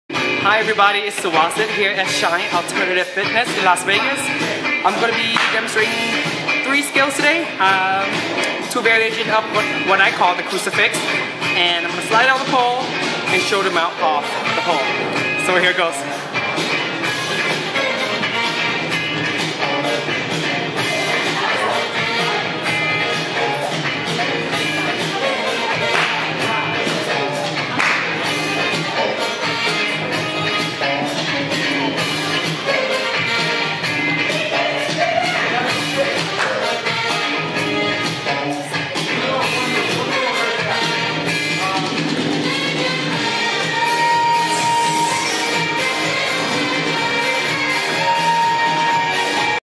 Suwasit's Crucifix
Two variations of "The Crucifix" on the pole. The first one is also know as a side hold plank. The second is also known as an upside down crucifix. I know that everyone around the world may call it something different.